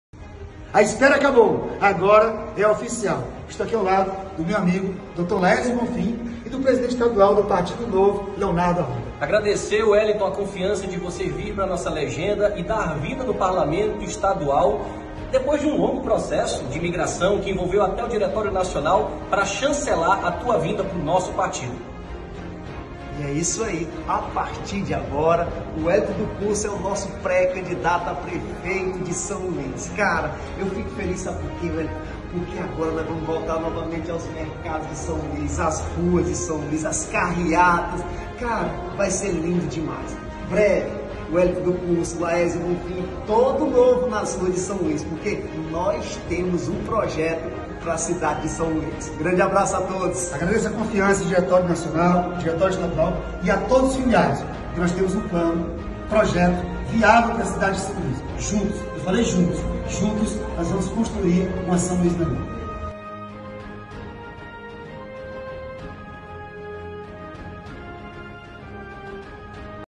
Wellington é confirmado oficialmente pré-candidato a prefeito de São Luís pelo partido Novo
Vídeo de blogdanielmatos